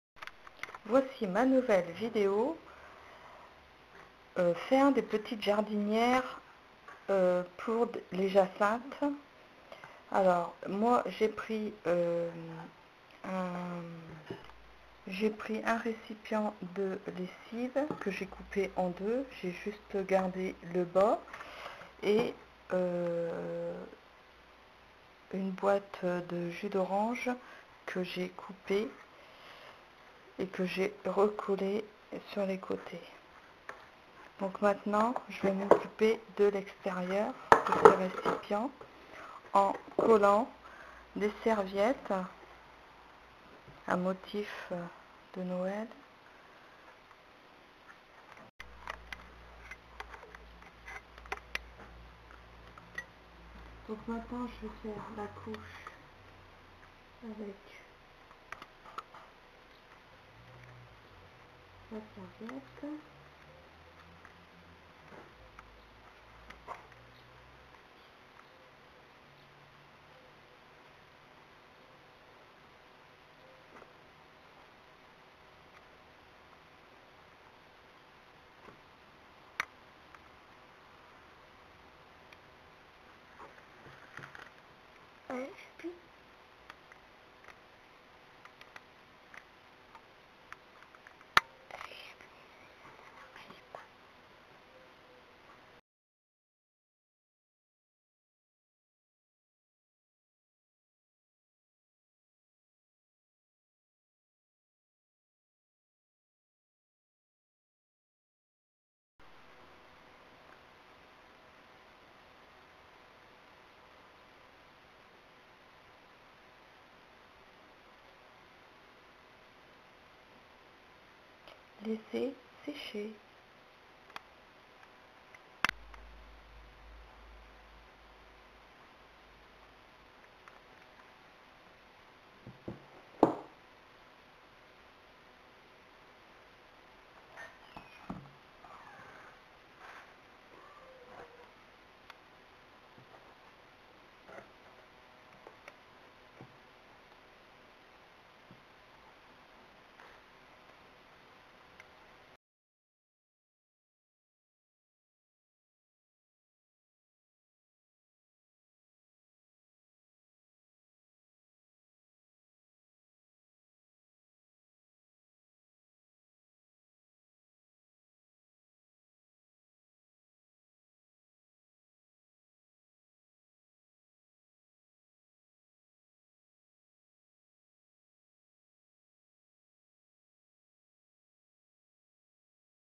pots pour jacinthes
faire des pots unique pour vos jacinthes ,décoration de table pour noel